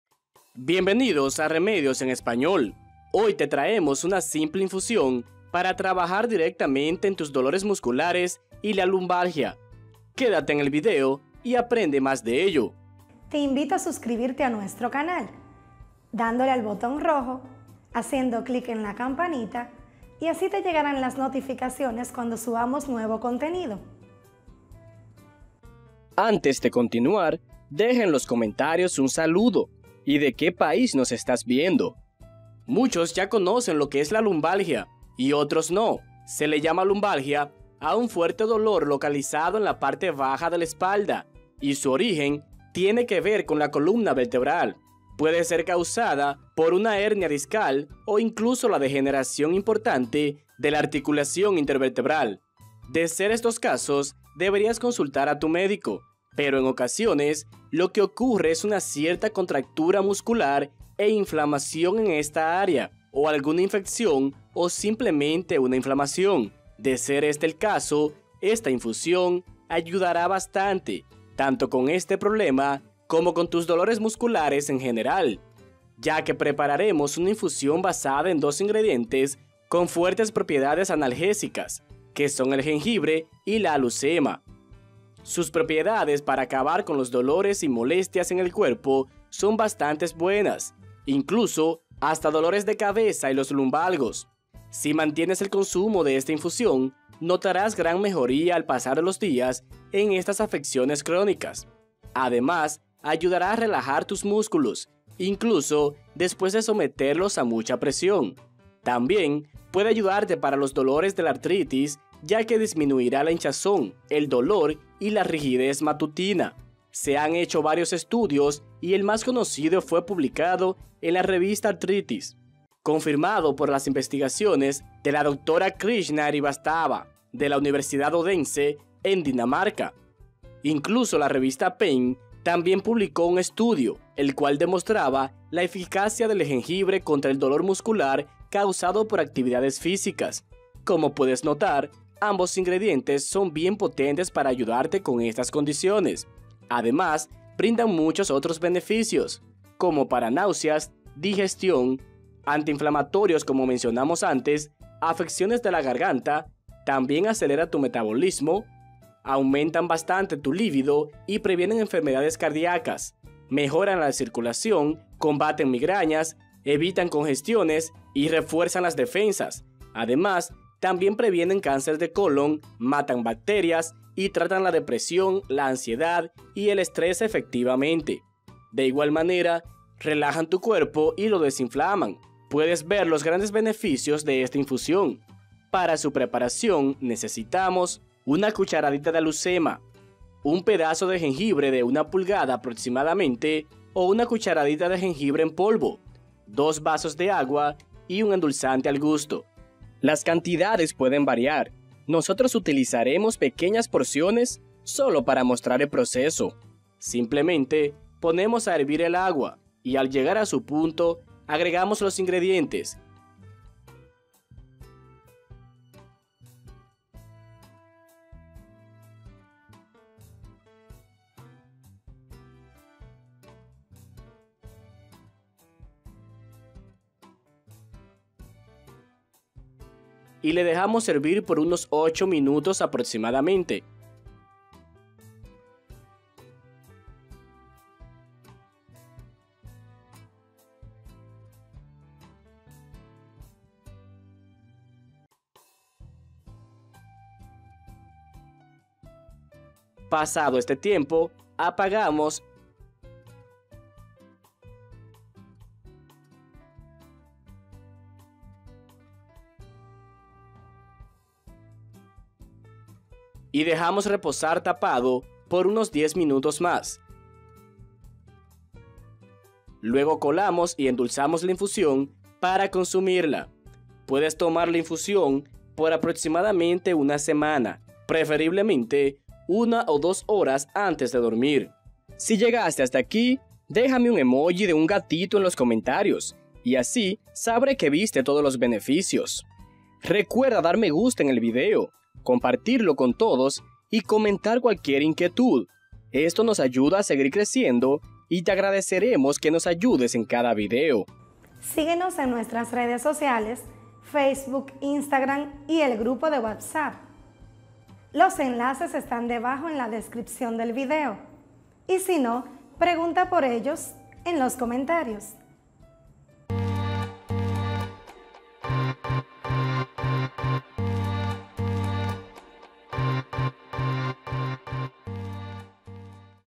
🟥 Alivia la LUMBALGIA desde Casa con este Remedio Casero 🍵 (Dolor Lumbar, Lumbago) 😃
Bienvenidos a Remedios En Español, hoy te traemos una infusión para trabajar directamente en tus dolores musculares y la lumbalgia,
quédate en el video y aprende mas de ello.

Ingredientes:

1 Cucharadita de alhucema

1 Pedazo de jengibre de una pulgada aproximadamente o una cucharadita de jengibre en polvo

2 Vasos de agua

1 Endulzante al gusto